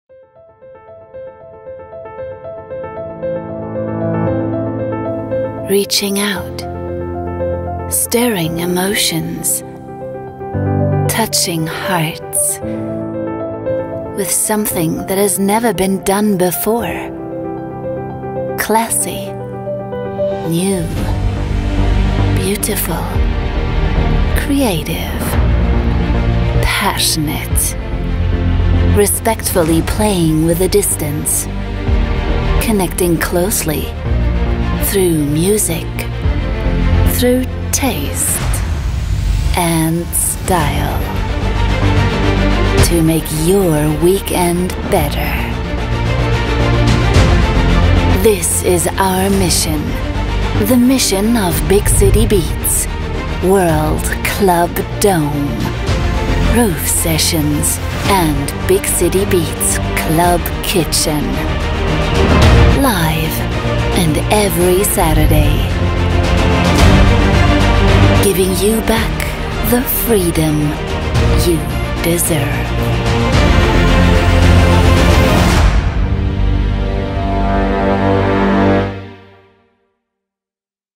WCD Roof Sessions #5 Villa Kennedy Aftermovie
As part of a unique BigCityBeats livestream series, we visited the well known Villa Kennedy for the 5th edition of the WorldClubDome Roof Sessions.
Check out how it was, when music and food, 2 of the greatest loves of human kind, connect.